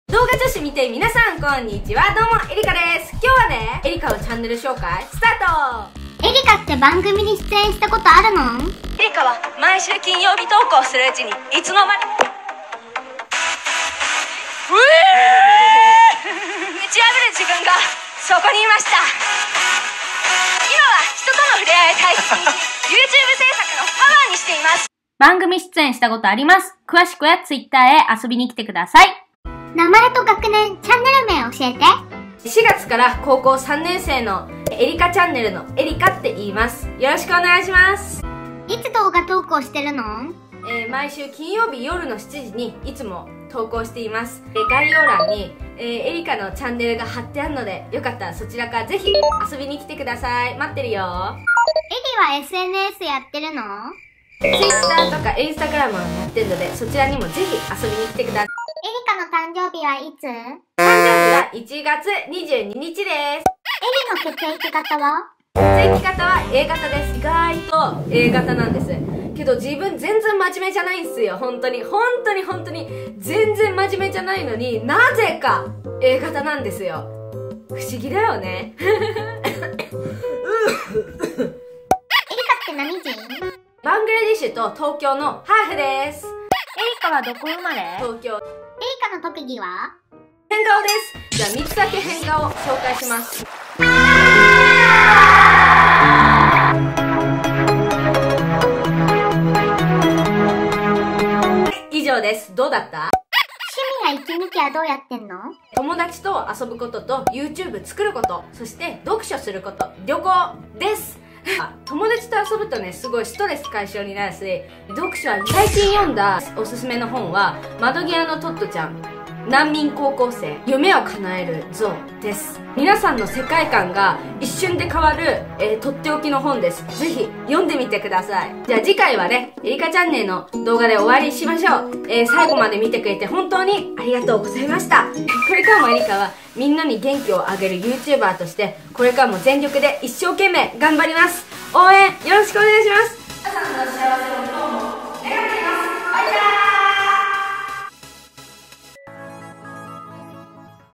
【えりかチャンネル】自己紹介+変顔やり放題！+マジおすすめの本3冊紹介📖👓
Amazon
【世界観が変わる本おすすめ3冊】
窓際のとっとちゃん
難民高校生
ゆめをかなえるぞう

【最近の日記】
桜🌸綺麗すぎる！！✨
【次回の動画は？】
毎週金曜日　夜7時投稿！